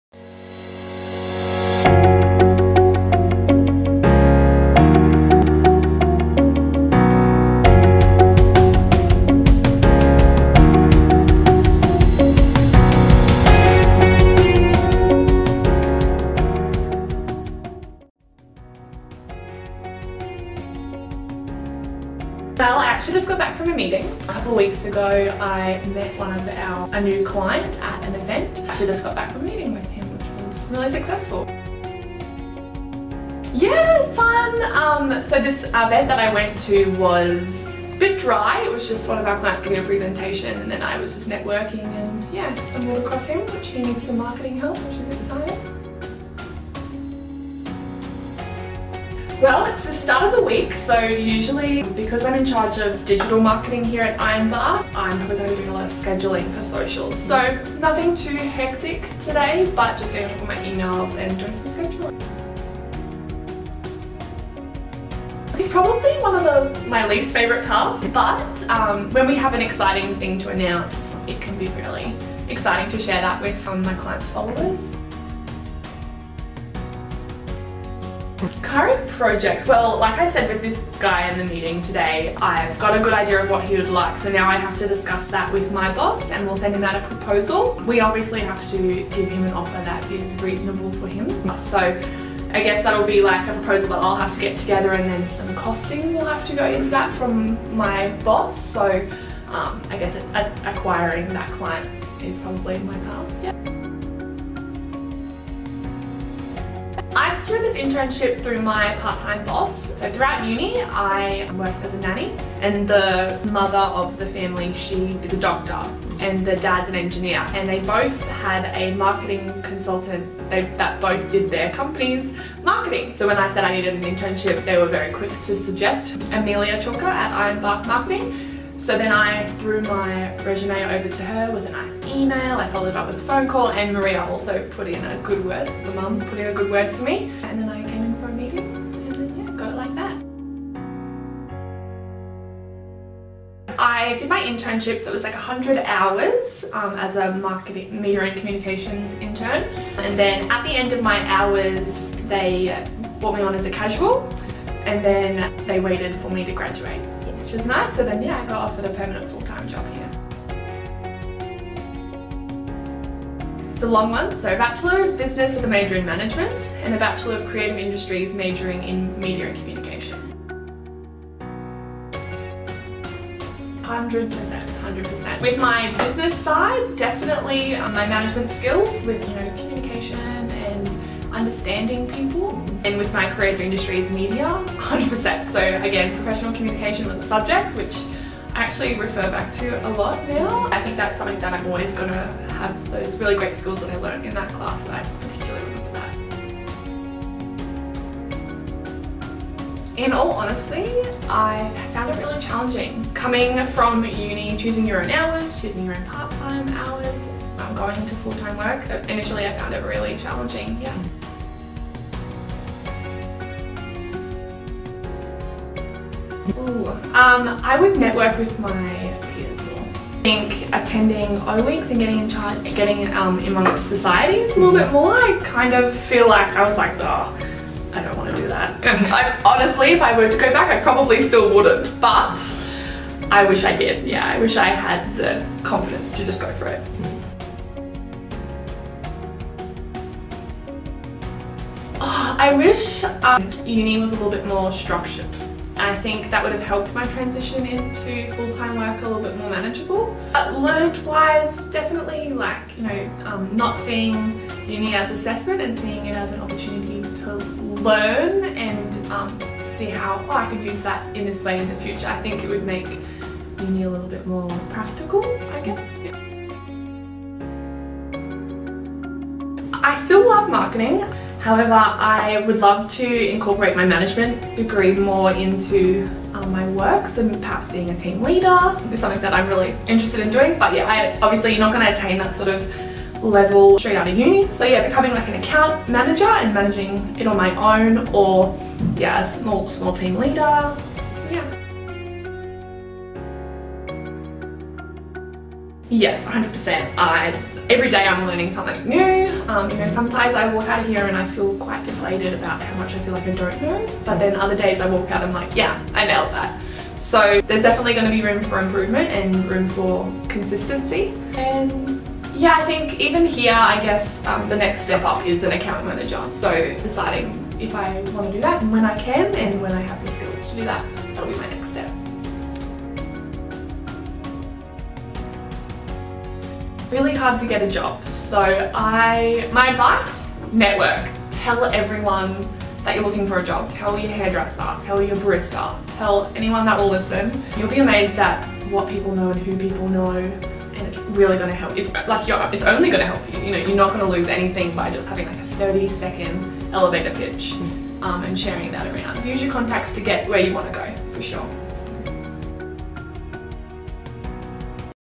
Interview with a QUT Graduate
Find out more about what we get up to here at MediaMap Brisbane!

Our MediaMap Brisbane team sat down with Ellie Blumel, a QUT graduate, to gain exclusive insight on her recent employment with Ironbark Marketing.